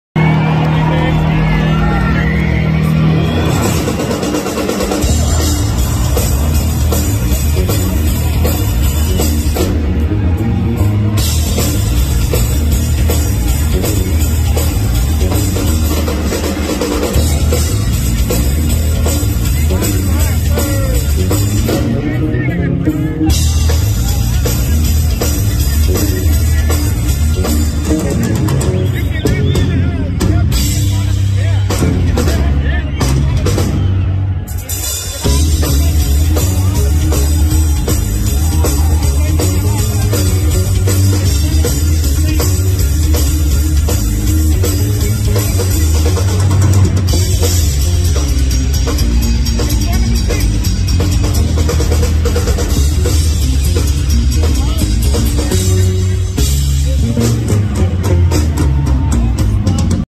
Mikkey Dee solo drums Last show Las Vegas 2022 @scorpions
Mikkey Dee solo drums 2022 Las Vegas . The Last Show!!!    Full video Mikkey Dee solo drums